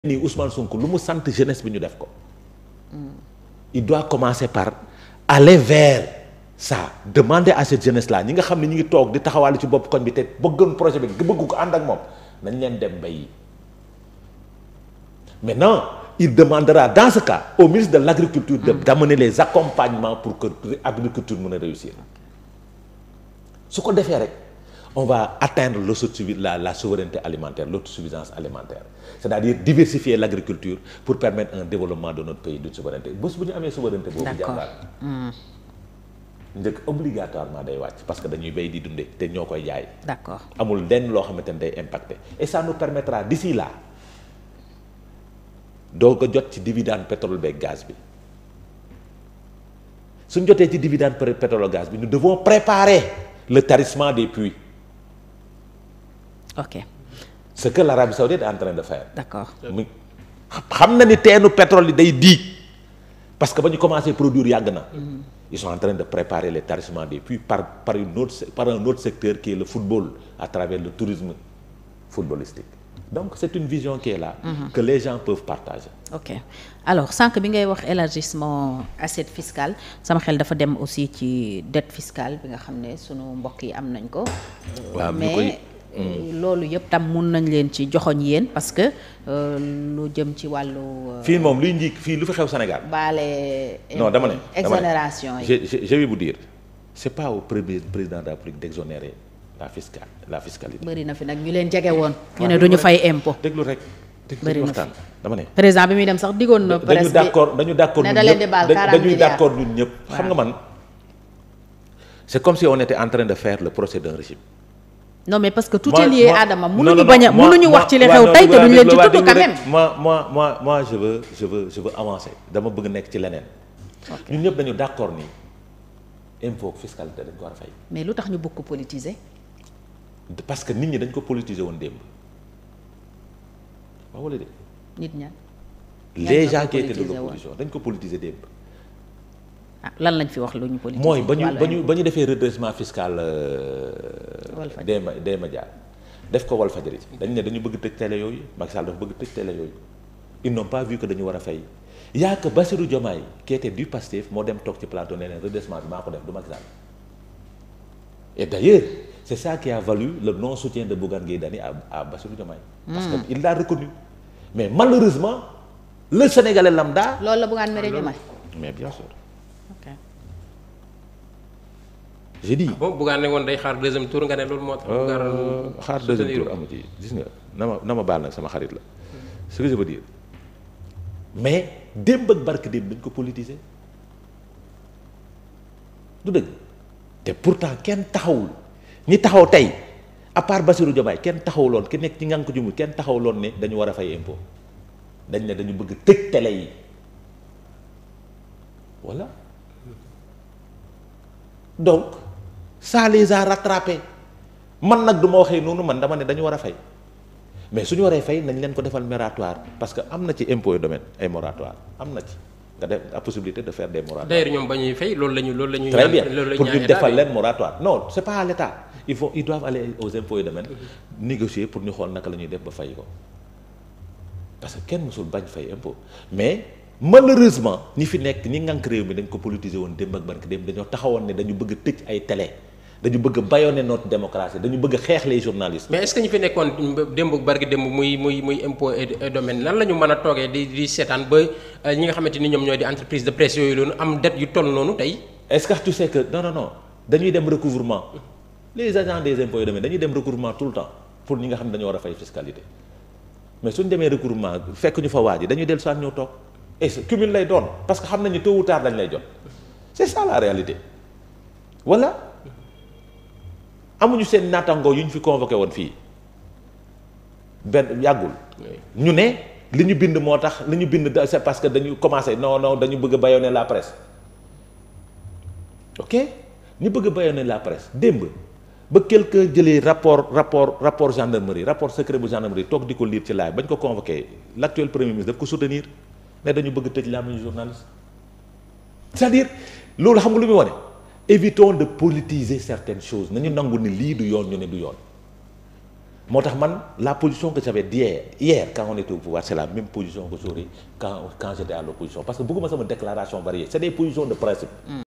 Problèmes entre Diomaye et Bougane:Adama Fall révèle "Bougane dafa Méré Diomaye ndakh...lolou motax
Les vérités de Adama Fall à Diomaye et SONKO, dette fiscale de la presse, Amadou Ba et APR, AFP...